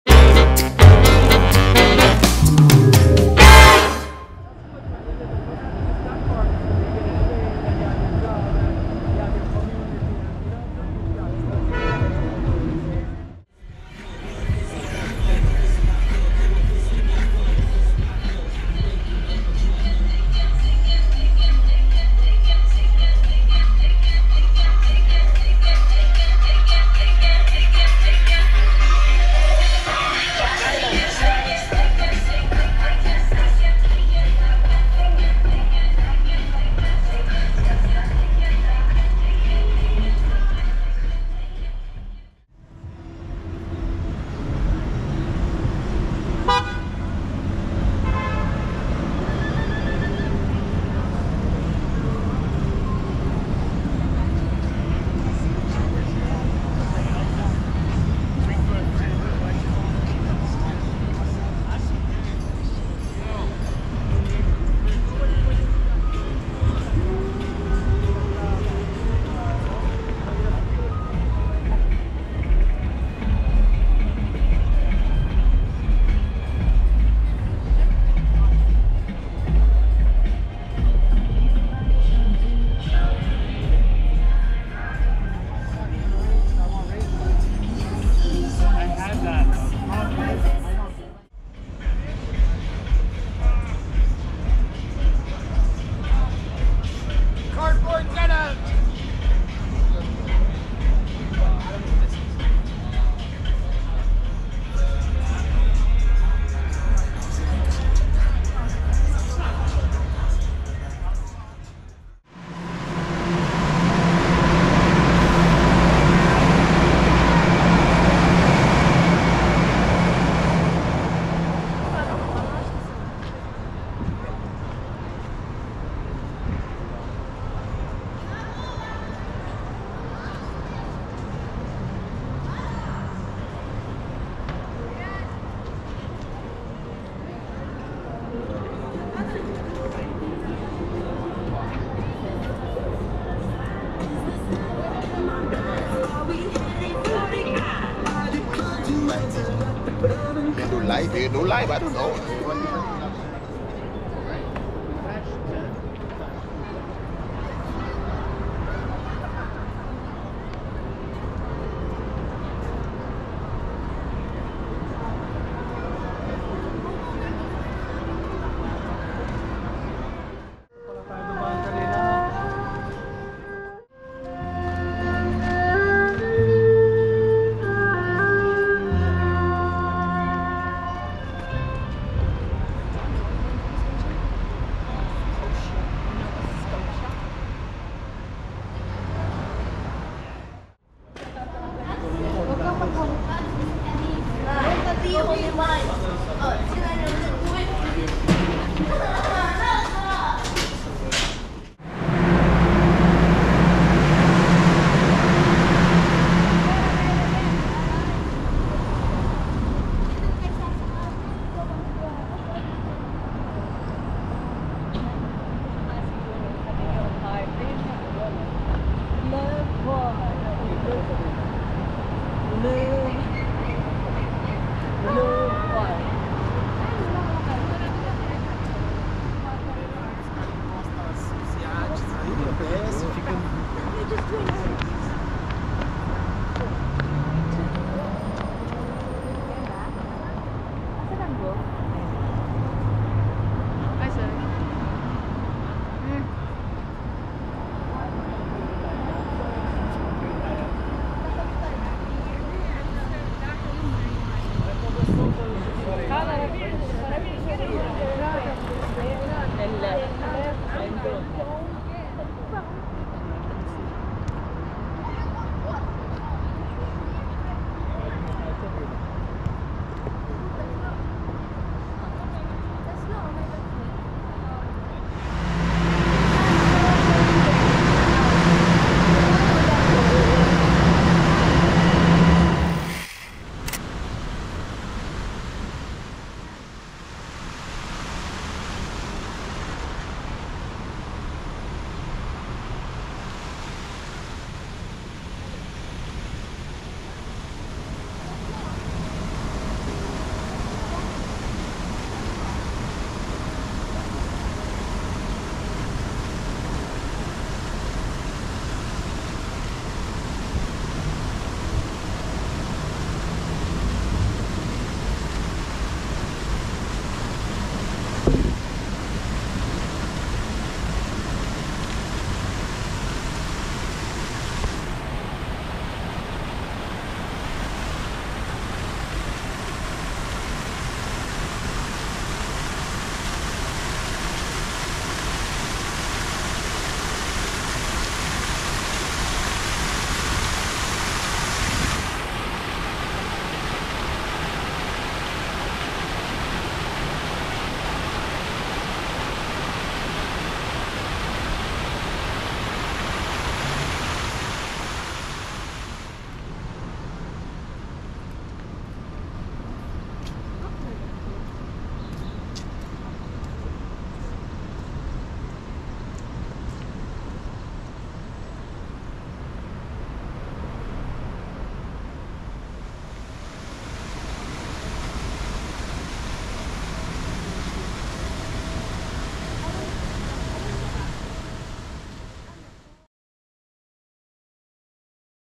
TORONTO | DOWNTOWN NIGHT WALK
TORONTO  DOWNTOWN LATE NIGHT SCENES

If you are wondering where to go out in Toronto at night, look no further than the city’s vibrant nightlife scene.

You will encounter a unique ambience when exploring Toronto at night. The city offers a diverse range of activities, including many bars, restaurants, clubs, and music venues. From stunning views of the skyline and a lit-up CN Tower, to live nighttime festivals and cultural events, there are plenty of fun things to do in Toronto at night.